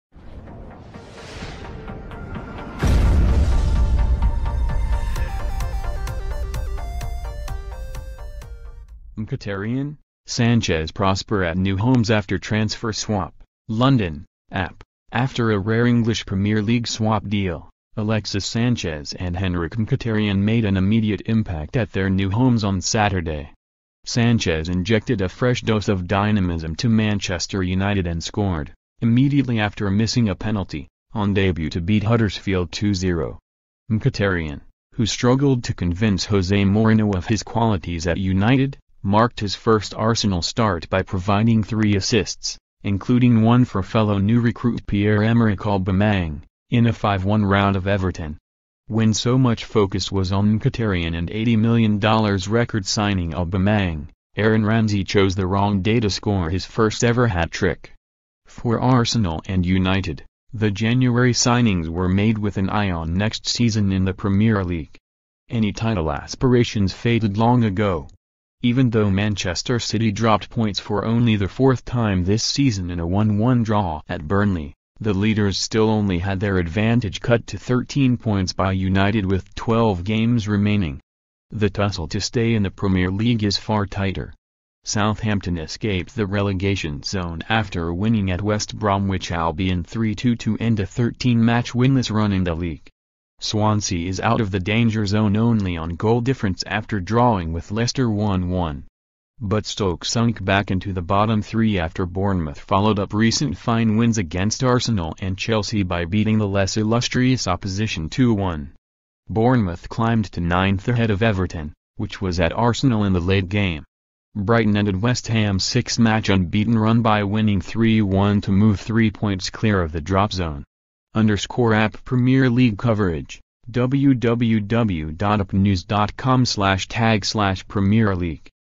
Mkhitaryan, Sanchez prosper at new homes after transfer swap
LONDON (AP) — After a rare English Premier League swap deal, Alexis Sanchez and Henrikh Mkhitaryan made an immediate impact at their new homes on Saturday.

Sanchez injected a fresh dose of dynamism to Manchester United and scored — immediately after missing a penalty — on debut to beat Huddersfield 2-0.

Mkhitaryan, who struggled to convince Jose Mourinho of his qualities at United, marked his first Arsenal start by providing three assists — including one for fellow new recruit Pierre-Emerick Aubameyang — in a 5-1 rout of Everton.